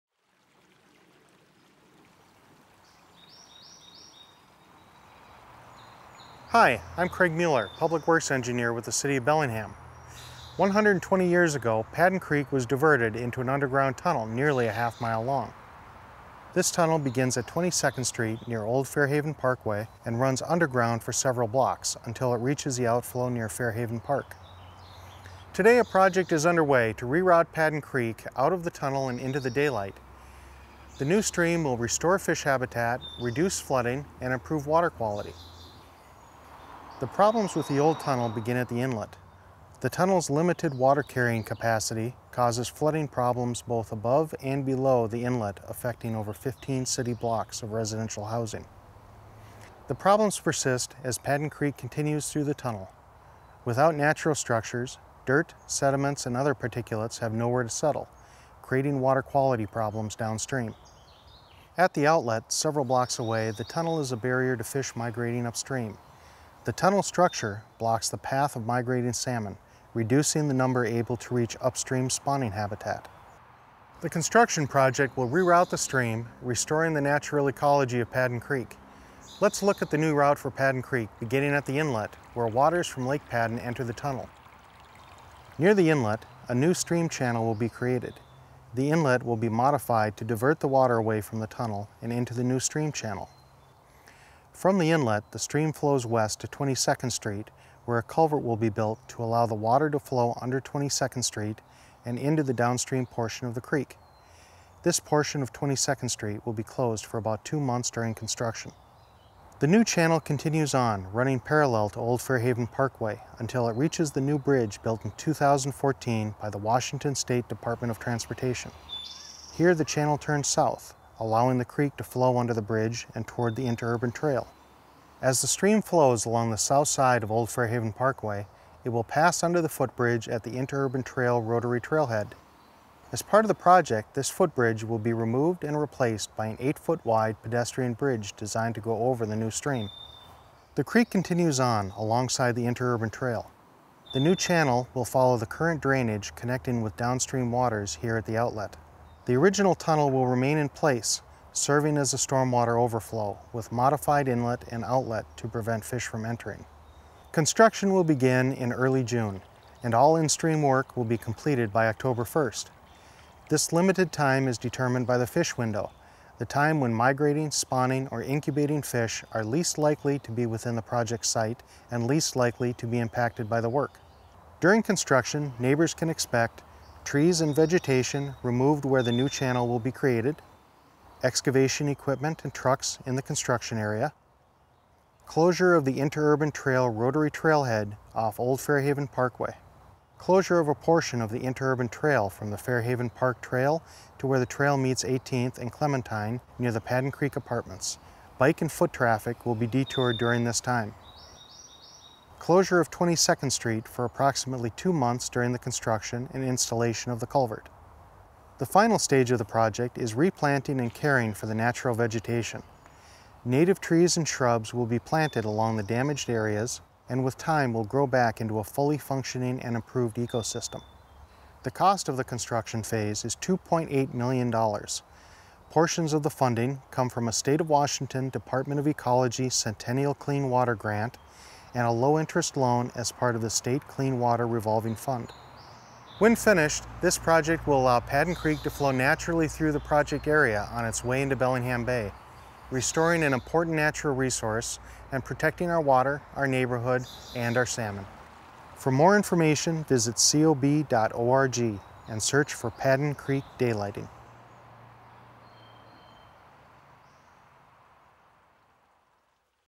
Padden Creek Daylighting_Part One
This project involves rerouting 2,200 feet of creek from the existing tunnel to a natural stream channel to allow fish passage, improve habitat and reduce flooding. The project will begin in June of 2015 and be completed in October of 2015.